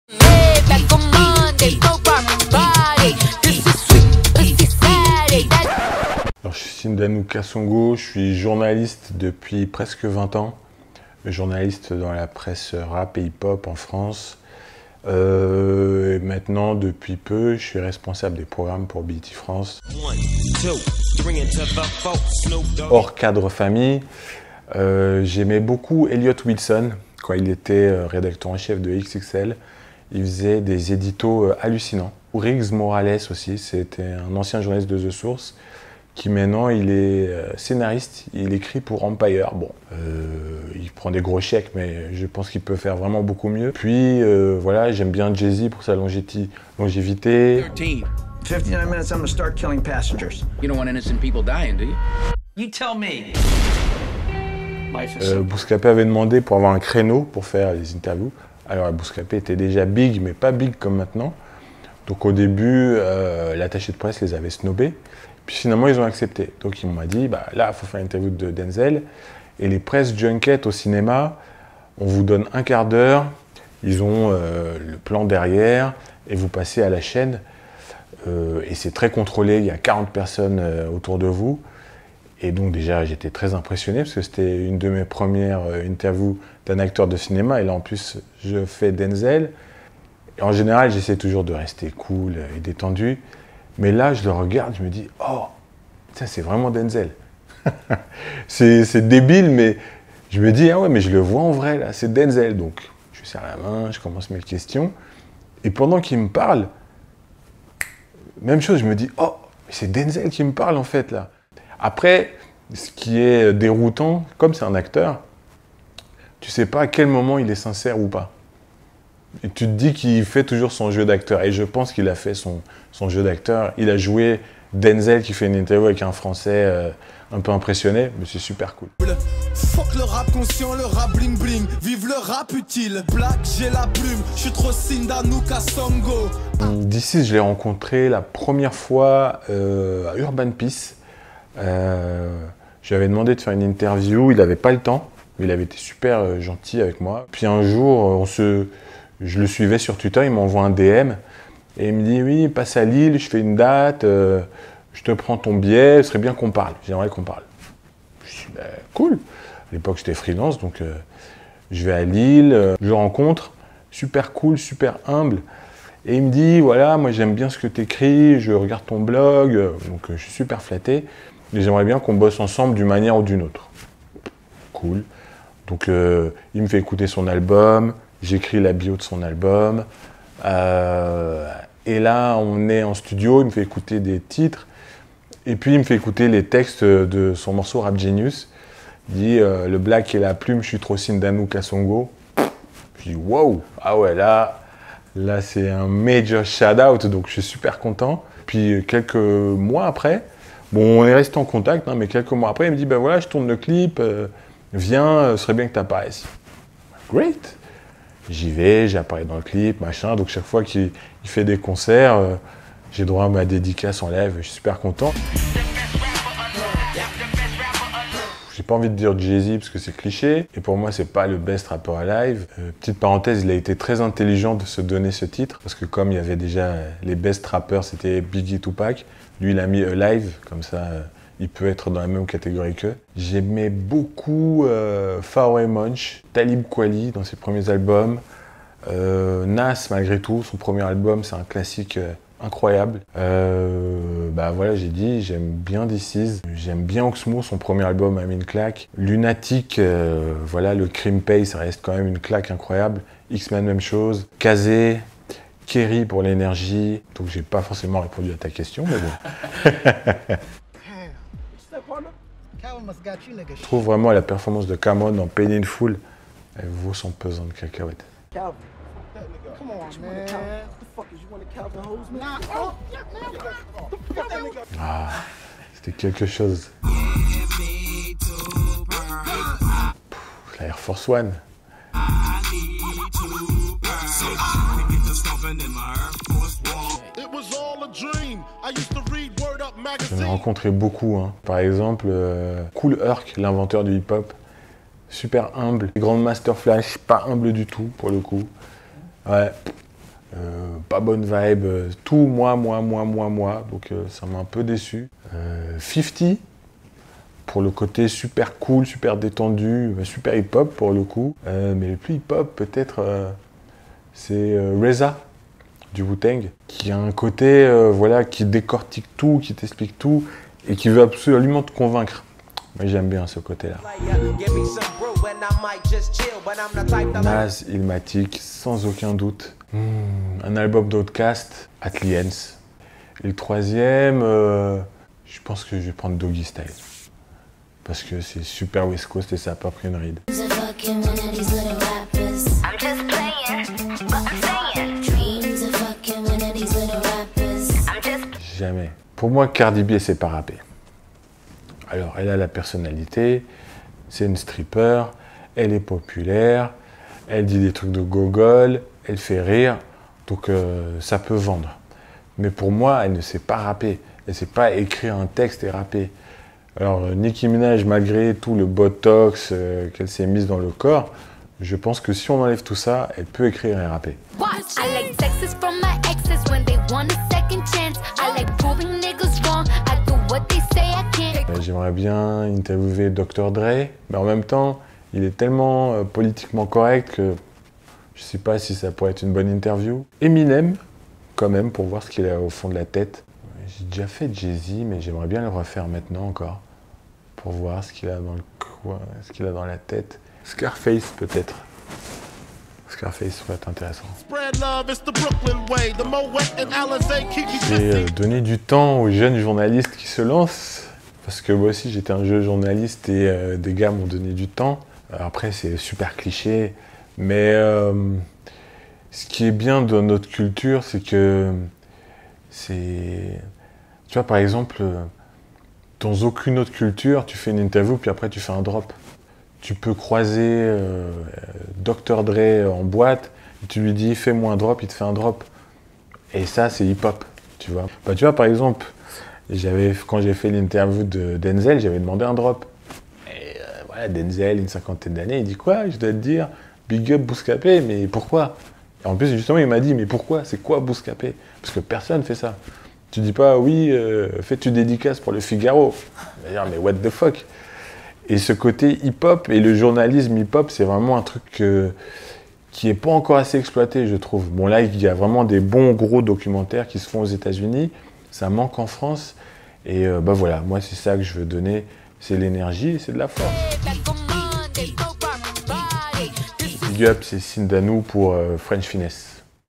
Sindanu Kasongo LFDL #01
Le Hip-Hop ne s’arrête pas seulement à ses MC’s mais également à tous ceux qui font vivre cette culture. A travers notre série « Les figures de l’ombre du Hip-Hop », nous allons mettre en avant ces personnalités qui contribuent ou qui ont contribué à l’avancée de cette culture. Et pour ce premier épisode, Sindanu Kasongo, directeur des programmes pour BET France, est notre invité. Avec près de 20 ans de carrière au sein de la presse Hip-Hop - Rap, Sindanu détient de nombreuses anecdotes en stock.